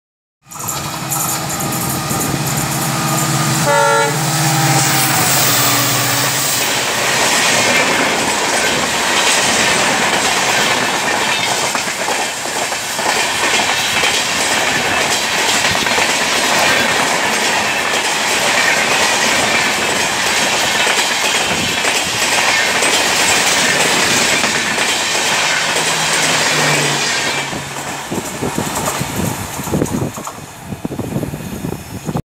Mumbai LTT - Karaikal Weekly Special
Special train from Mumbai LTT to Karaikal
Stops - 29
Duration - 30 hrs 30 minutes